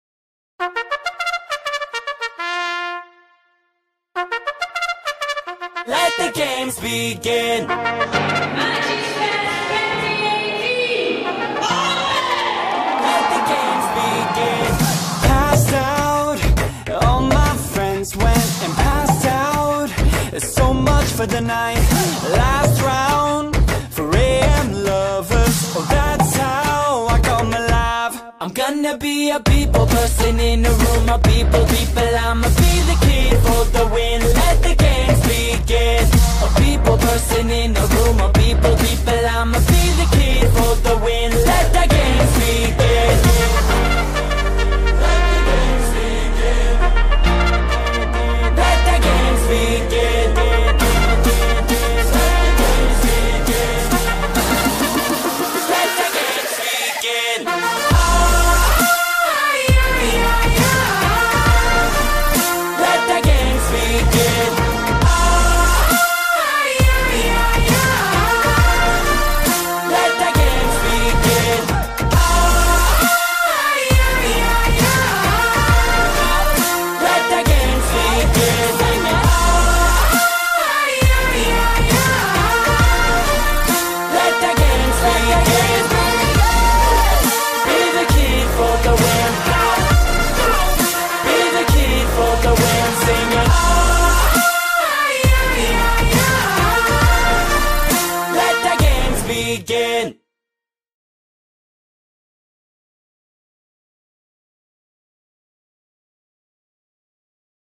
DAY 1 - Highlights of Magis Fest 2018
SHS-Ateneo de Cebu MAGIS FEST
2018 (Senior High School Intramurals 2018 DAY 1)

DAY 1 HIGHLIGHTS:
Soccer, Volleyball & Basketball
Extemporaneous Speech Competition
Art on Wheels